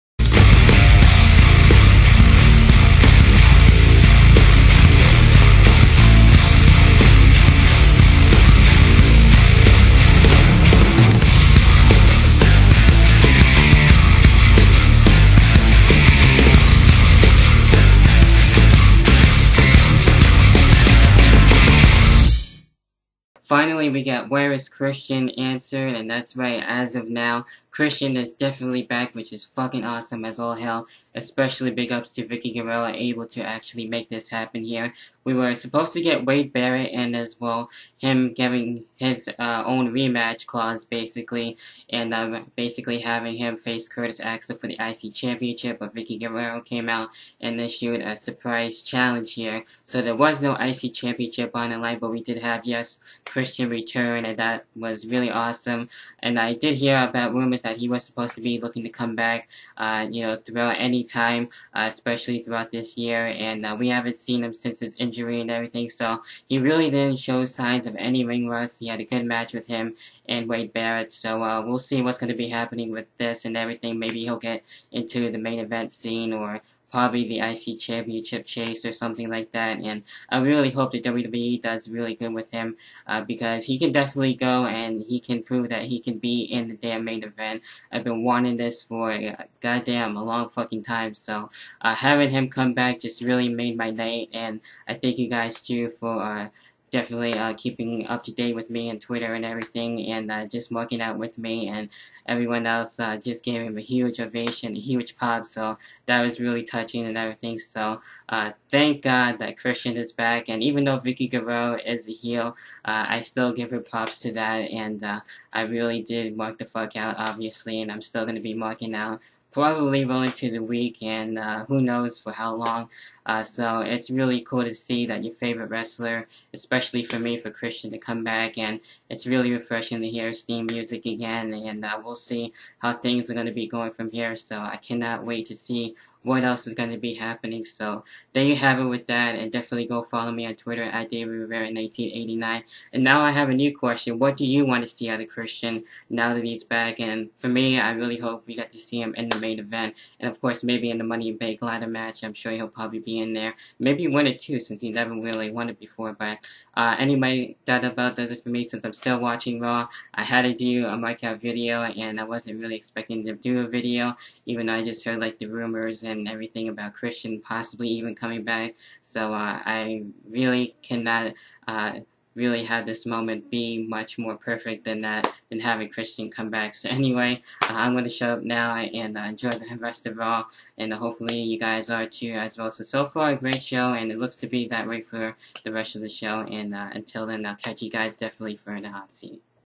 Breaking News : Christian Returns To The WWE On Raw 6/17/13 Mark Out Thought's And Opinions
Breaking News: Christian Returns To The WWE On Monday Night Raw 6/17/13 Thought's And Opinions And Mark Out Video From A Christian Fan.